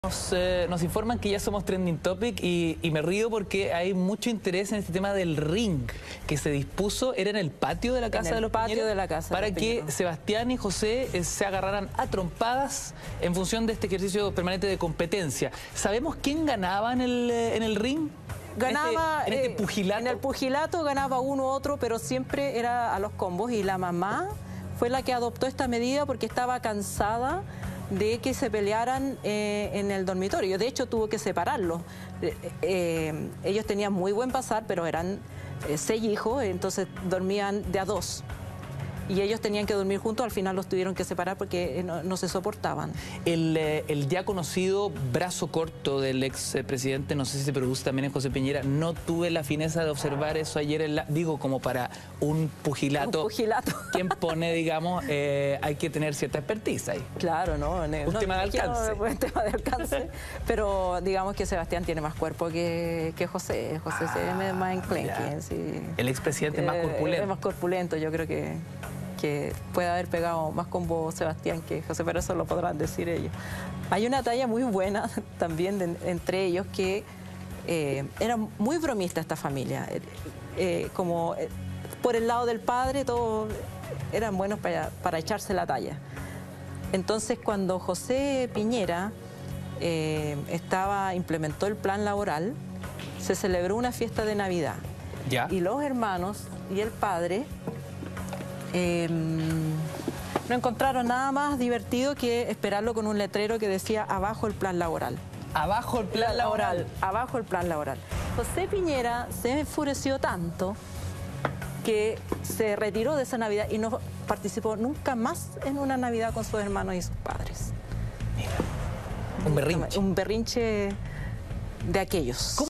Las impactantes peleas que tuvieron Sebastián y José Piñera en su infancia
Alejandra Matus comentó acerca de estas peleas que tenían los hermanos y que la madre de ellos estaba cansada, así que tomó la decisión de crear un ring.